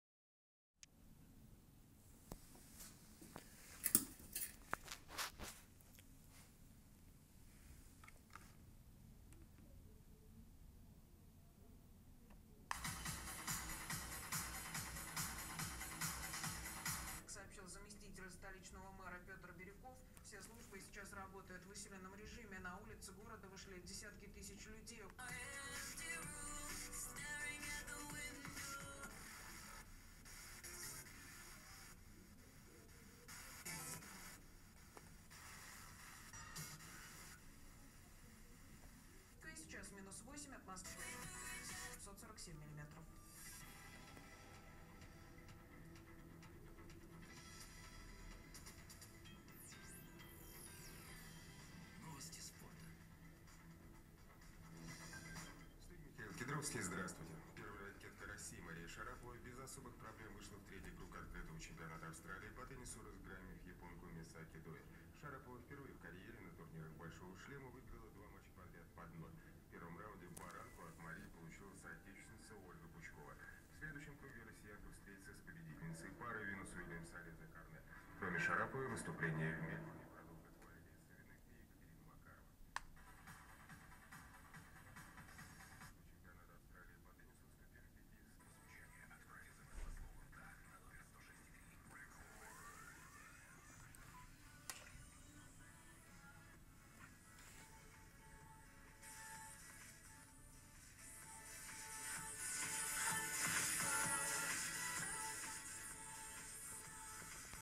FM tuner and audio mixer test. Second demo
Audio control for chery tiggo (m11, b11) headunit via android device. Bridge is teensy 2.0 mcu. Mux control for audio sources, fm-radio control. At last cut-off headunit front panel and replacing it winth android tablet. Own opensource software for android (now it is based project)